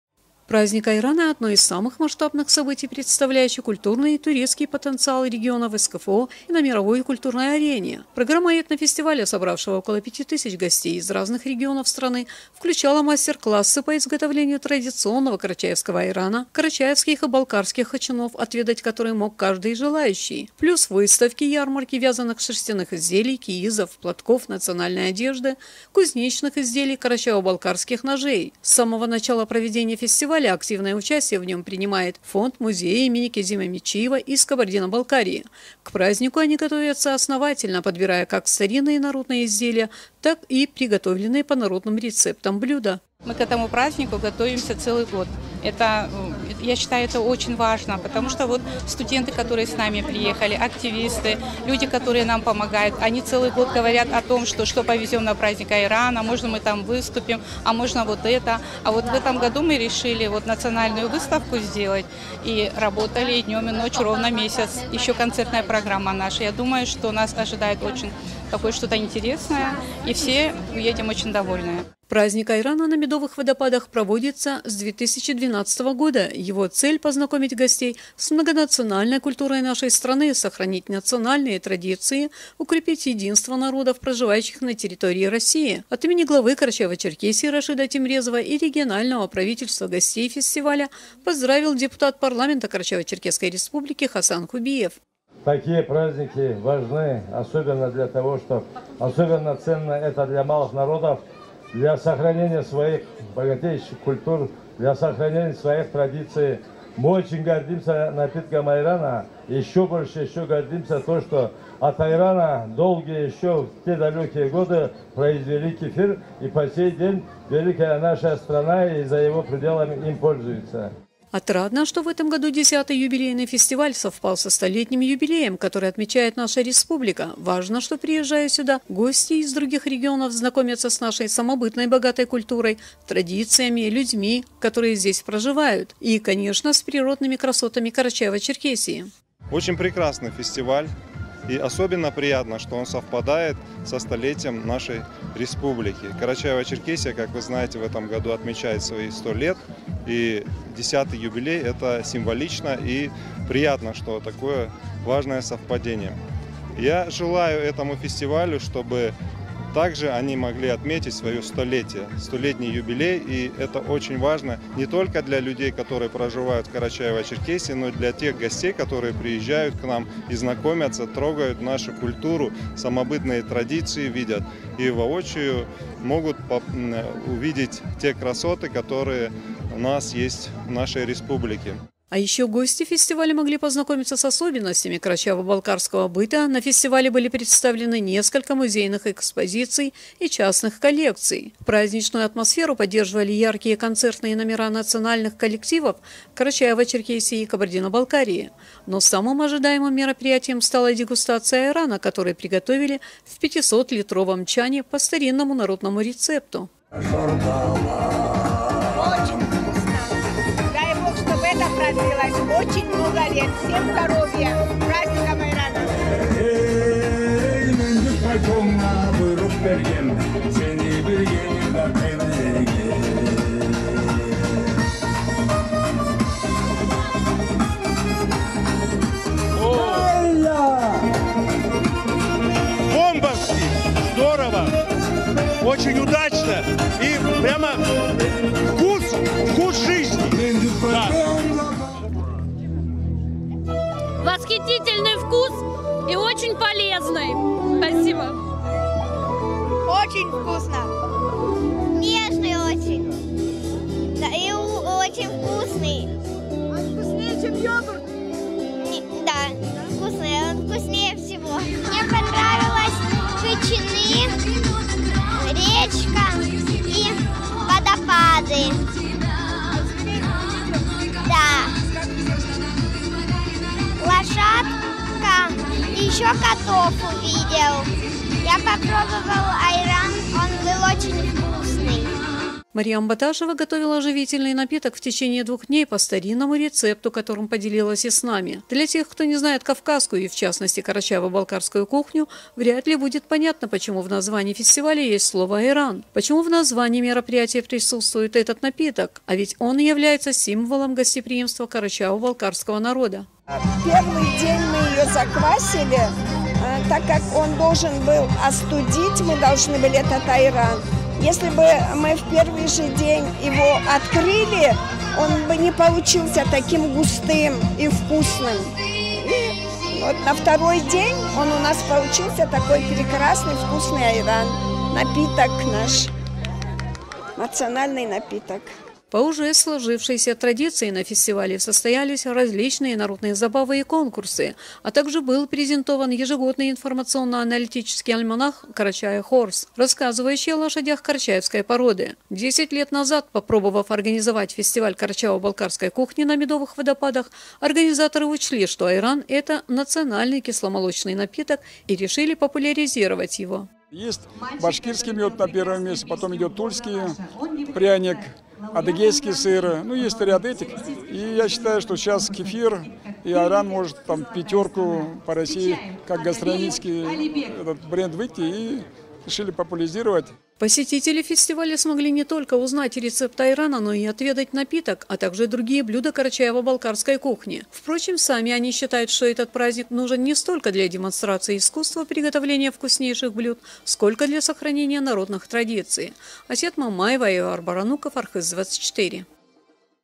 Праздник айрана на Медовых водопадах
Вот уже 10 лет при поддержке Главы КЧР Рашида Темрезова в нашей республике проводится «Праздник айрана» на Медовых водопадах. А в этом году юбилейный фольклорно-этнографический фестиваль культуры карачаево-балкарского народа еще и совпал со 100-летием республики. За время своего существования этот кисломолочный праздник приобрел статус международного, ведь его с удовольствием посещают гости из-за рубежа. Не в последнюю очередь ради знаменитой дегустации айрана, который готовят в 500-литровом чане по старинному народному рецепту. О празднике целительного напитка в сюжете .